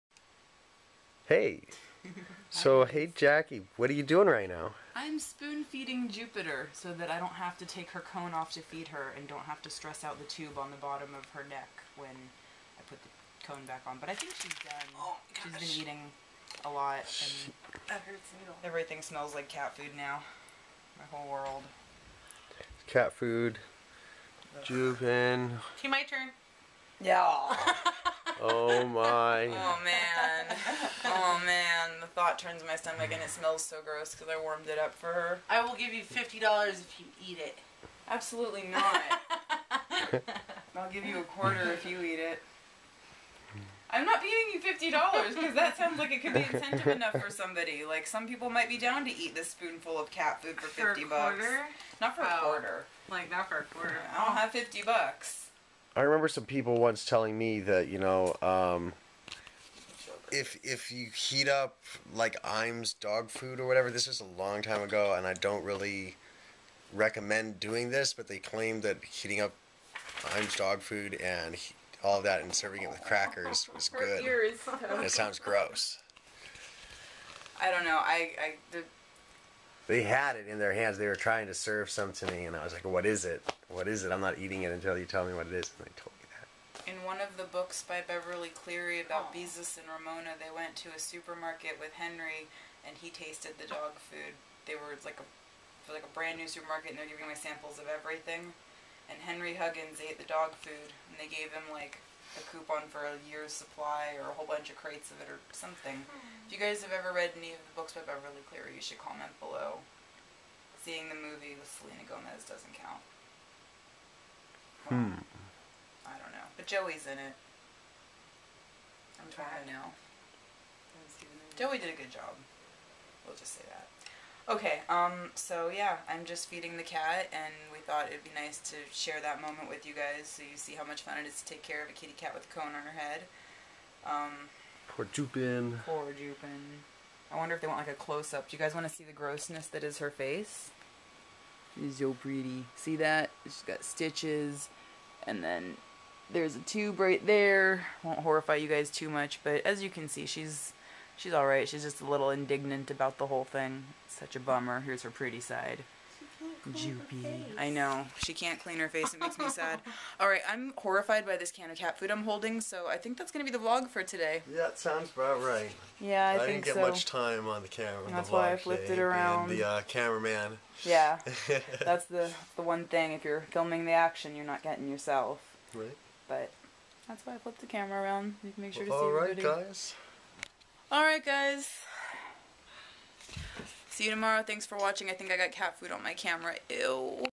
day 805 FEEDING JUPITER!!!
Hi Guys! Happy Wednesday!! Here's Jupiter having dinner in her cone!